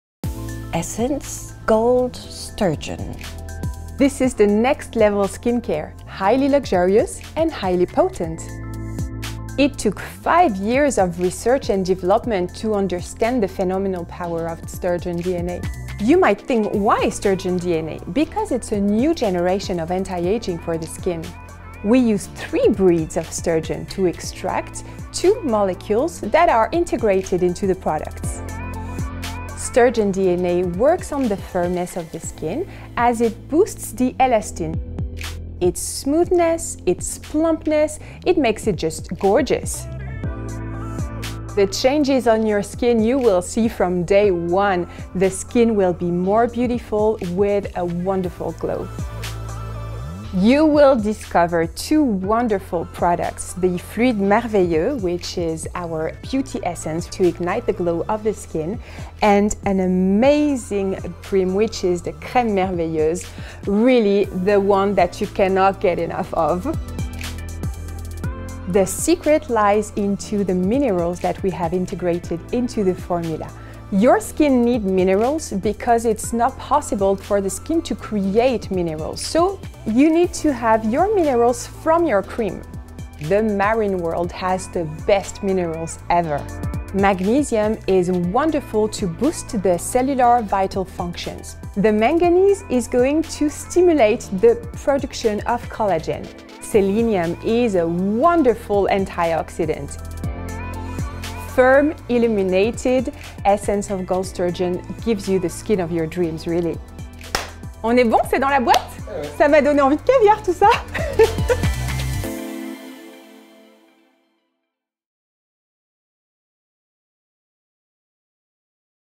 Essence of gold sturgeon complex
Are you ready for the consecration of beauty?

Valmont global education director, Lisa Mammeri, walks us through Essence of Gold Sturgeon ingredients.

Essence of Gold Sturgeon by l’Elixir des Glaciers enhances the unrivalled illuminating virtues of marine minerals, coupled with intense elastin benefits of sturgeon DNA. And your firmed skin glows from within.
Because beauty deserves its consecration.